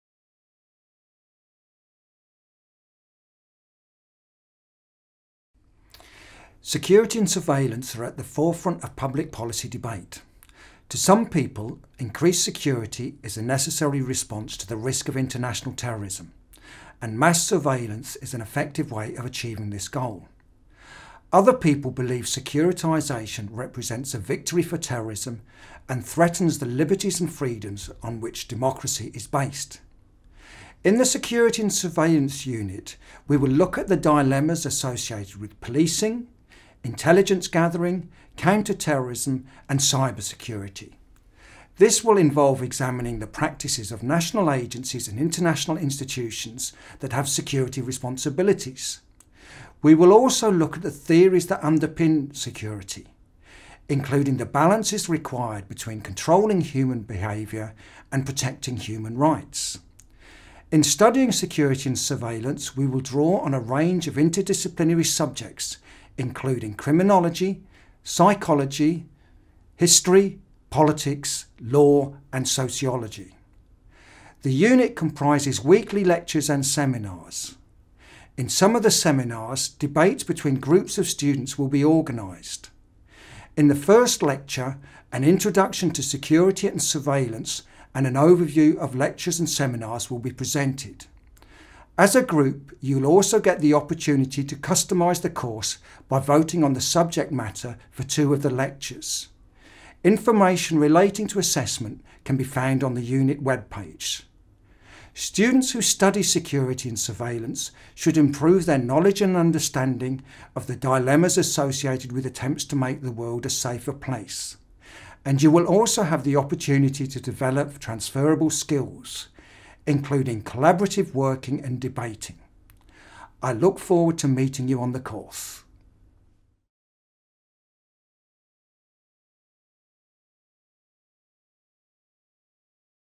Security and Surveillance
In this video, course convener Dr Graham Smith discusses the University College course Security and Surveillance. He discusses what students will learn and what they will get out of undertaking this course unit.